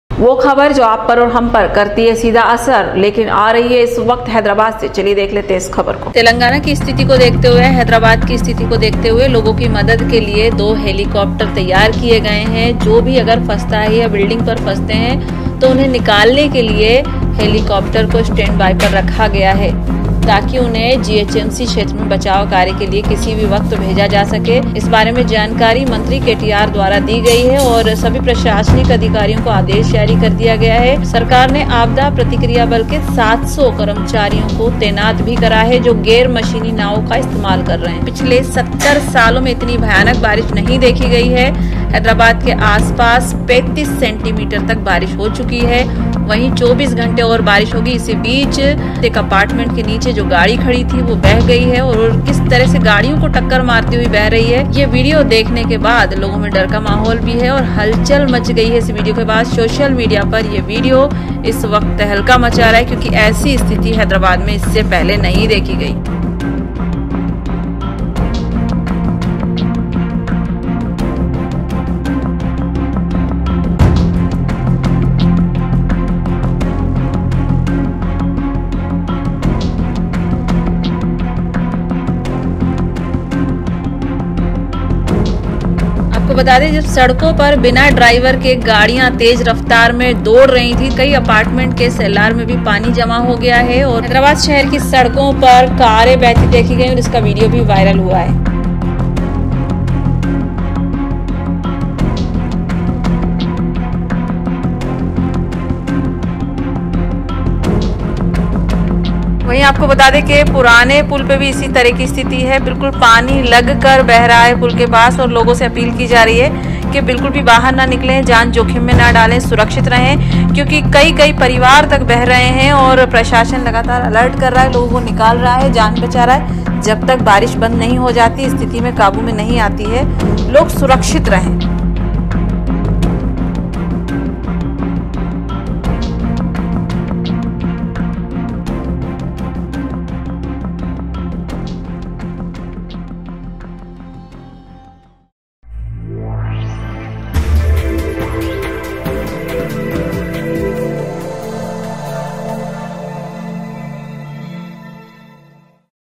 Hyderabad Floods // भारी बारिश Cars Flowing in Water, Viral video
Hyderabad Floods // भारी बारिश और बाढ़, नाव की तरह बहने लगी कारें // Cars Flowing in Water